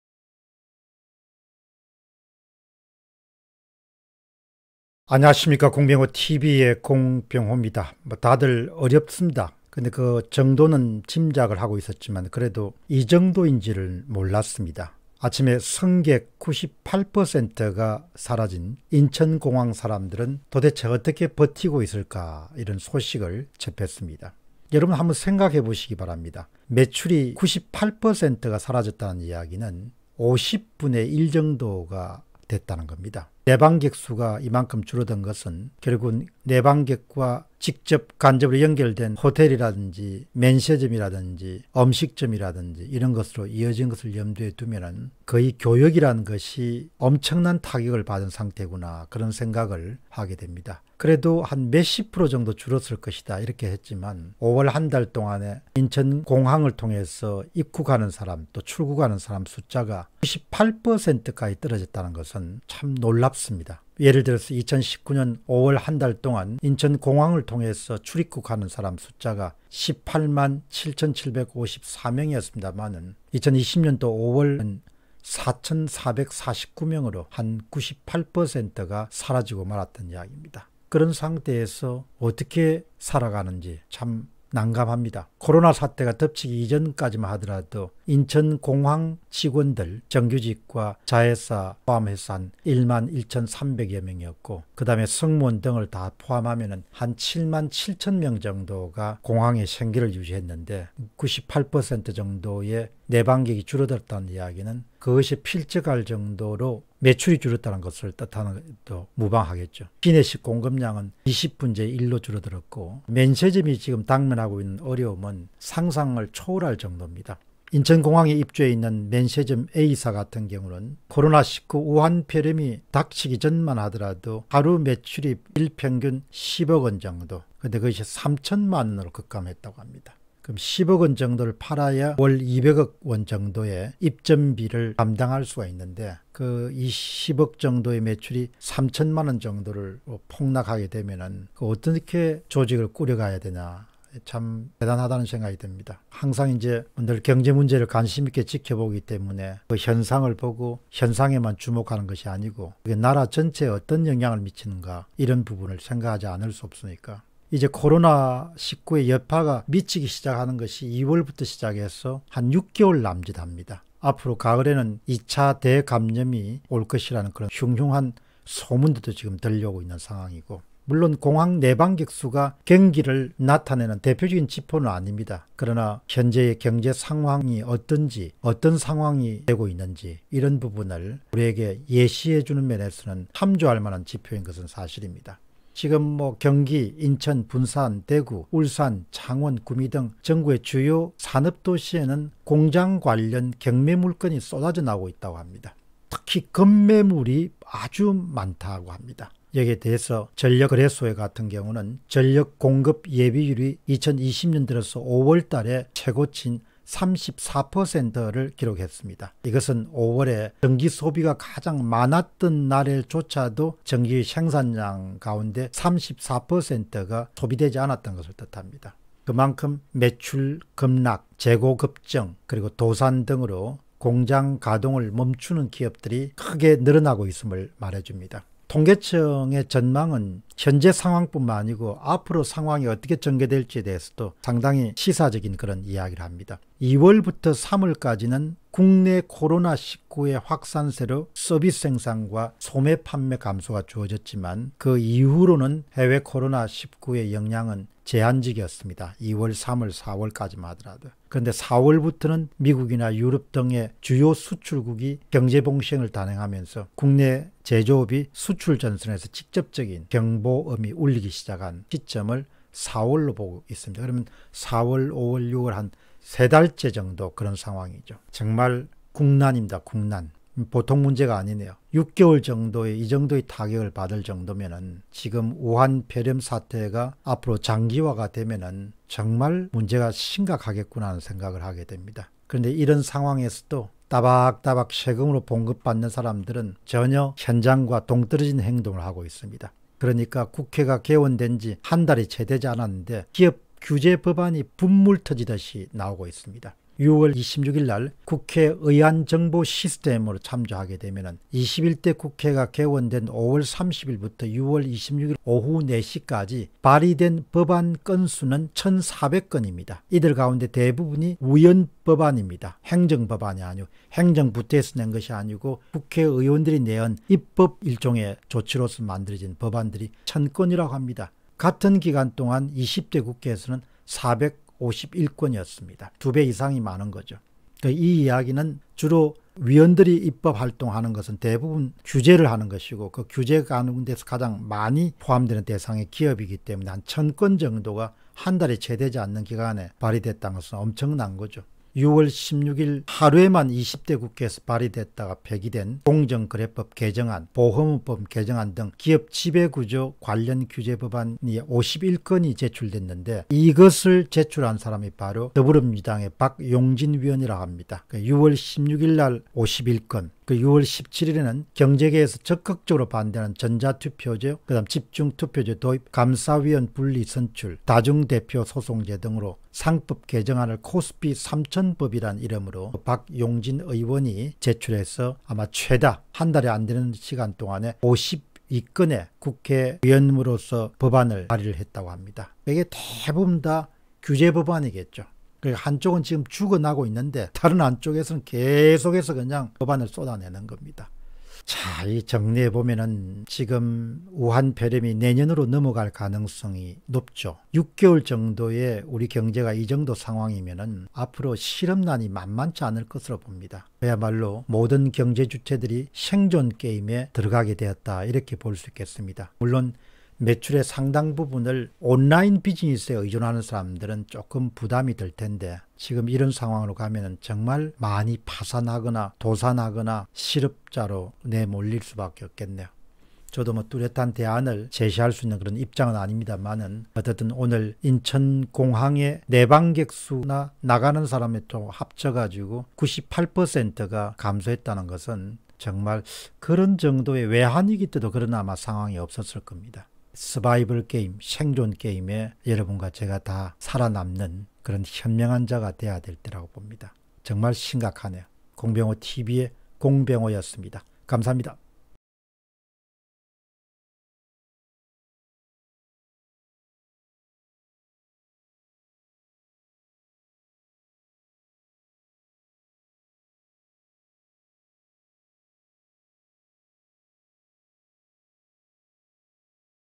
지금 경제상황 / 이 정도일 줄이야 [공병호TV]
Bitcoin : 3MTW849AkP2ftLeSK2teZHMmzLu7Z83s9U

참고자료
1. 승객 98%가 사라졌다, 인천공항 사람들 어떻게 버티고 있지?, 곽창렬, 조선일보, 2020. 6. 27
2. 팬데믹 ‘불황 거미줄’에 숨막혀…공장 경매 물건 쏟아진다, 황종일, 중앙일보, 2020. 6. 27
3. 21대 국회 한달 ... 규제 퍼죽 법안 쏟아졌다,